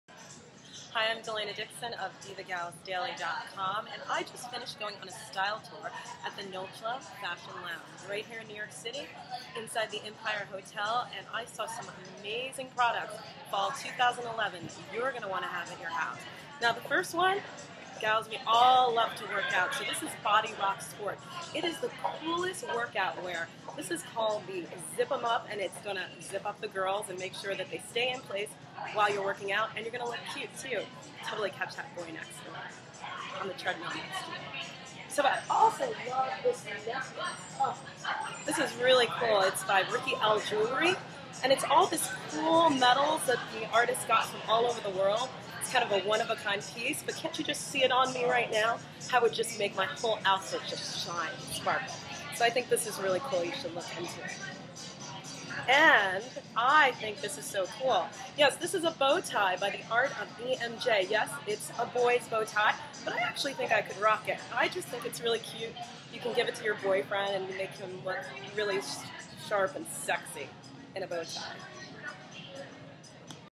Style Tour Coverage from DivaGalsDaily at The Nolcha Fashion Lounge
Delaina Dixon, from DivaGalsDaily, shares what caught her eye during her Style Tour at the Nolcha Fashion Lounge.